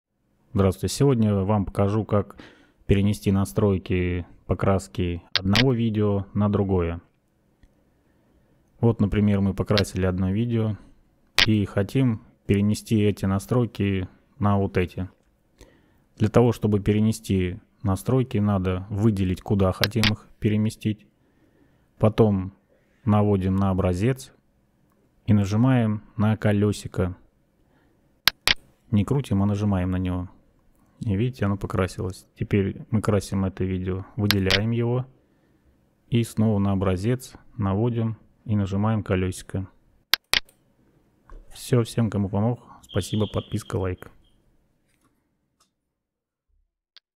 Перенос настроек покраски видео в Davinci Resolve
Здесь показано как легко перенести настройки покраски видео на другое видео в давинчи резов во вкладке color.